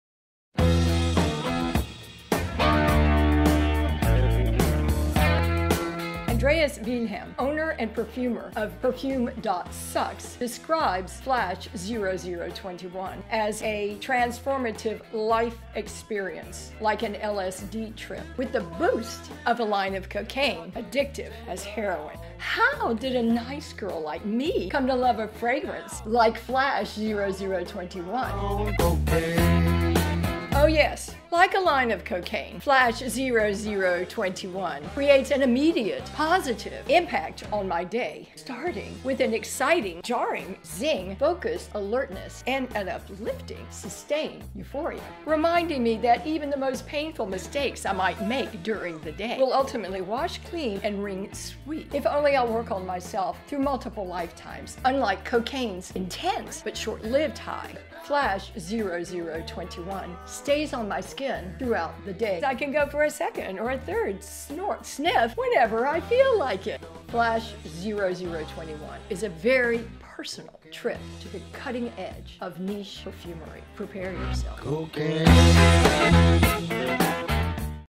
Flash0021 by Perfume.Sucks
Why do I love Flash0021 by Perfume.Sucks ?
It's Uranus energy.
Wild. Untamed.
Portending drastic change.
Out of time.
The spirit of a new age.
Coming on us fast.
Physical and abstract.
A unique and ephemeral conjunction of multiple time-lines
In a never-to-be-repeated moment.
Triggered by the collision of
*A real and physical chemical formula
*My skin
*My unconscious self
 *My memories
*My dreams
*My plans for the day.

Andreas Wilhelm, Perfume.Sucks brand owner and perfumer, describes Flash0021as a life changing fragrance, like an LSD trip with the boost of a line of cocaine. Addictive as Heroine.

For me Flash 0021is at the cutting edge of niche perfumery.
Ready to go there?
Nobody is. And yet it’s here and now.
You’ve just gotta make the leap.
You might never come back.
And if you do, you might never be the same.

Can’t say I didn’t warn you.
It’s my job.
Xxx
Sarah and the Bad Girls
at Bad Girls Perfume.com

PS: Don’t worry. You’ll still stay BAD.